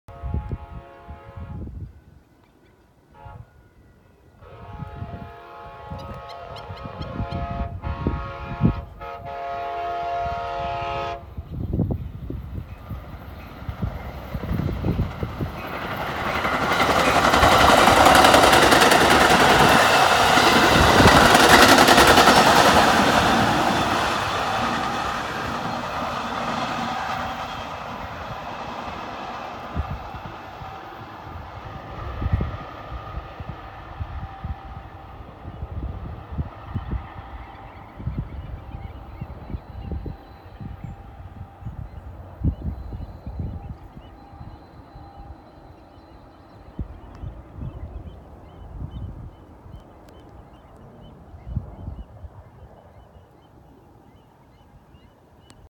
Alviso Amtrak
Amtrak Capitol Corridor going through the Alviso marina area